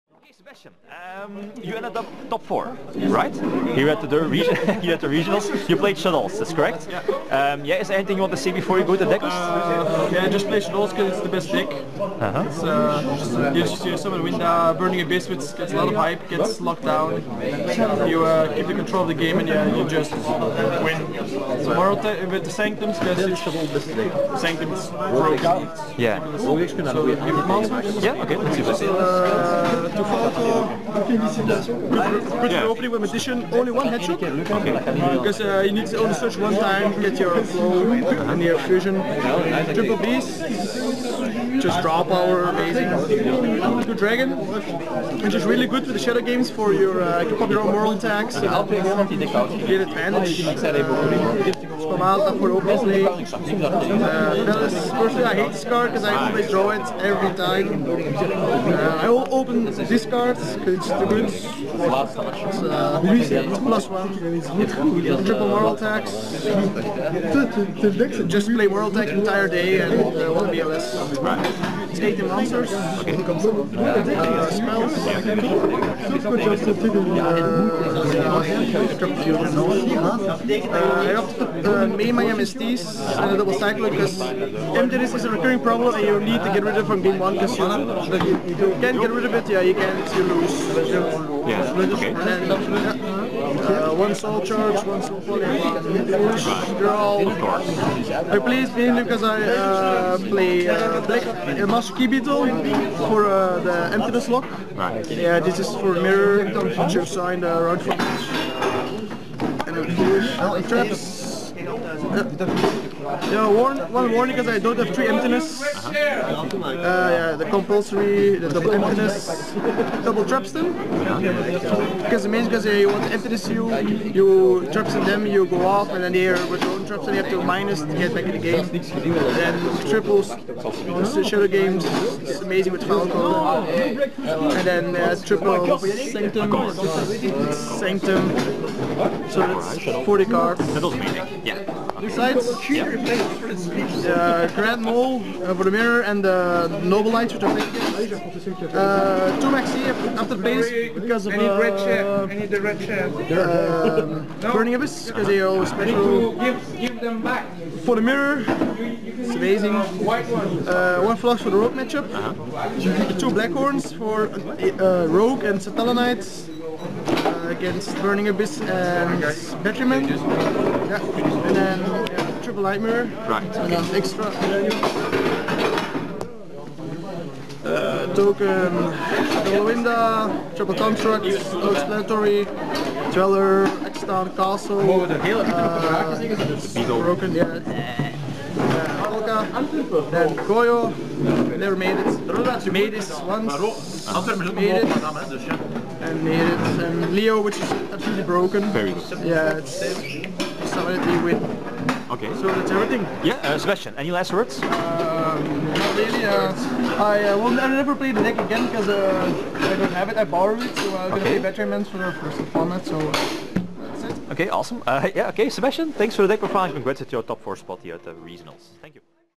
Yu-Gi-Oh! Deck Profile - WCQ Regionals - Top 4 - ‎Sebastian Faes - Shaddoll Artifacts
New Format (Oct. 1st 2014) - Top 4 - Shaddoll Artifacts by Sebastian Faes..